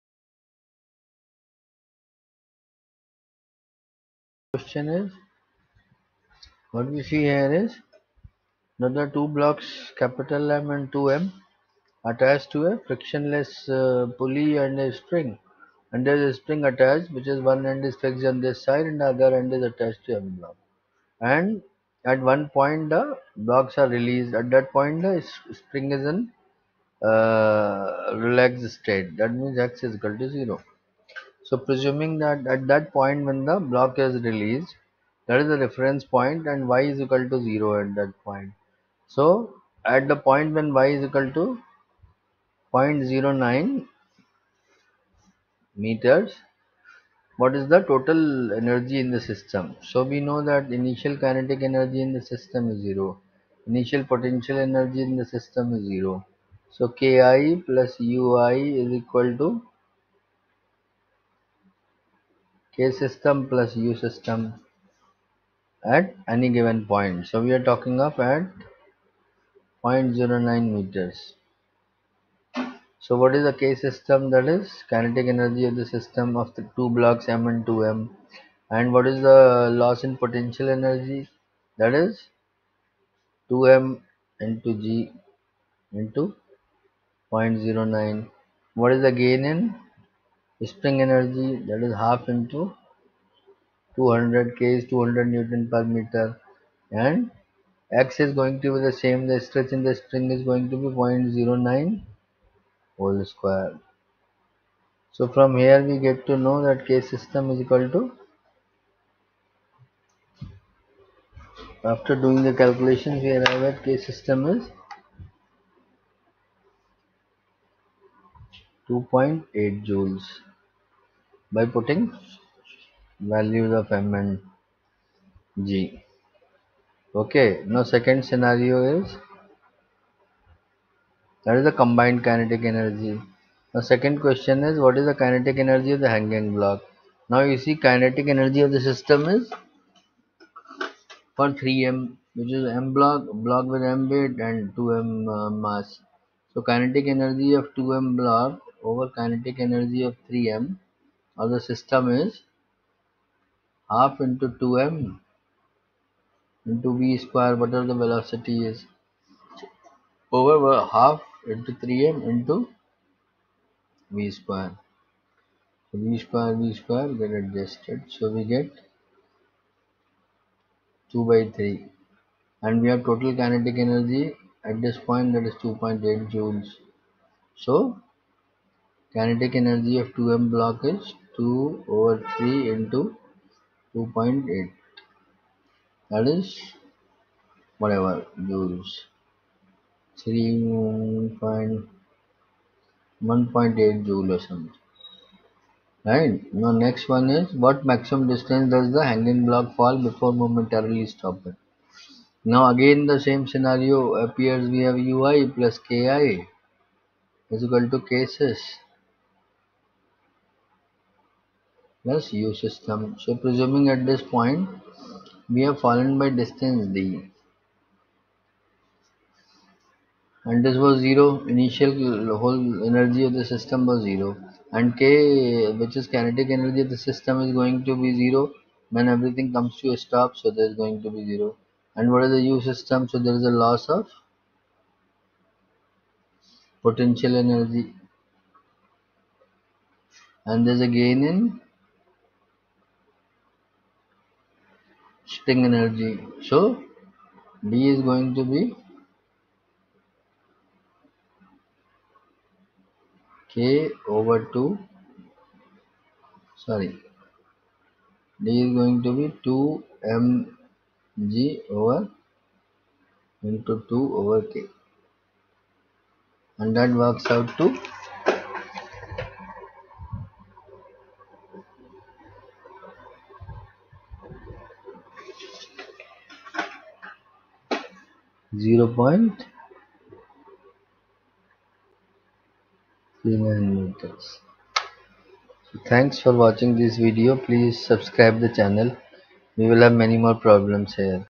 Kinematics : Energy in the System : Potential Energy
8.91 Two blocks, of masses M 2.0 kg and 2M, are connected to
a spring of spring constant k  200 N/m that has one end fixed, as
shown in Fig. 8-69. The horizontal
surface and the pulley are friction-
less, and the pulley has negligible
mass. The blocks are released from
rest with the spring relaxed.
(a) What is the combined kinetic
energy of the two blocks when the
hanging block has fallen 0.090 m?
(b) What is the kinetic energy of
the hanging block when it has
mum distance does the hanging block fall before momentarily
stopping

Fundamentals of Physics
Halliday & Resnick
10th Edition
Jearl Walker
JEE
Physics